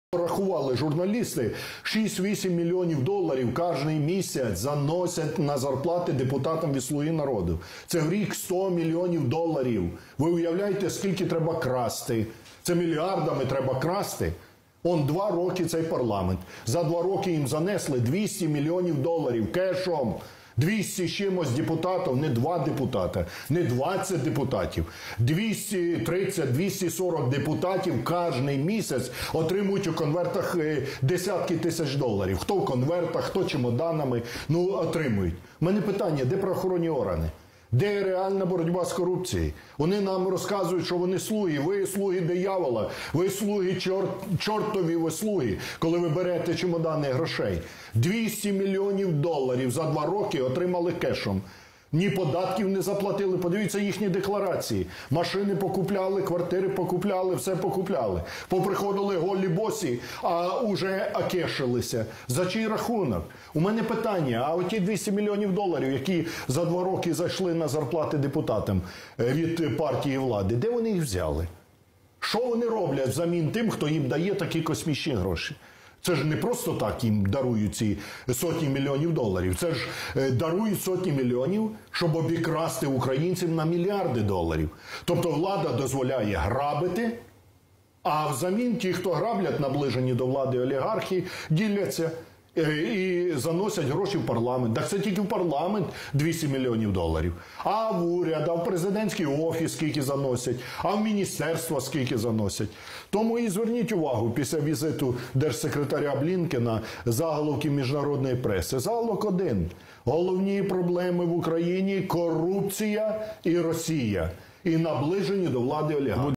«Слуги диявола, а не народу!». Ляшко про корупцію у парламенті
За два роки депутатам від «Слуги народу» занесли 200 мільйонів доларів зарплат готівкою, - ці дані журналістського розслідування вкрай обурили Олега Ляшка. В ефірі «Україна 24» політик заявив: про корупцію в парламенті всі знають, але ніхто не притягнутий до відповідальності.
«Журналісти порахували, що 6-8 мільйонів доларів щомісяця заносять на зарплати депутатам від «Слуги народу». В рік – 100 мільйонів доларів. За два роки, поки вони при владі – це 200 мільйонів доларів», - обурився Ляшко.
Лідер Радикальної партії переконаний, що ці гроші - українців.
«Де вони взяли за 2 роки 200 мільйонів доларів на зарплати депутатам? Що вони роблять взамін тим, хто дає такі «космічні» гроші? Не просто ж так їм дарують сотні мільйонів доларів. Їм дають ці гроші, щоб обікрасти українців на мільярди доларів. Тобто, влада дозволяє грабувати, а наближені до влади олігархи діляться. І це тільки в парламенті. А ще є уряд, офіс президента та все інше», - наголосив Ляшко.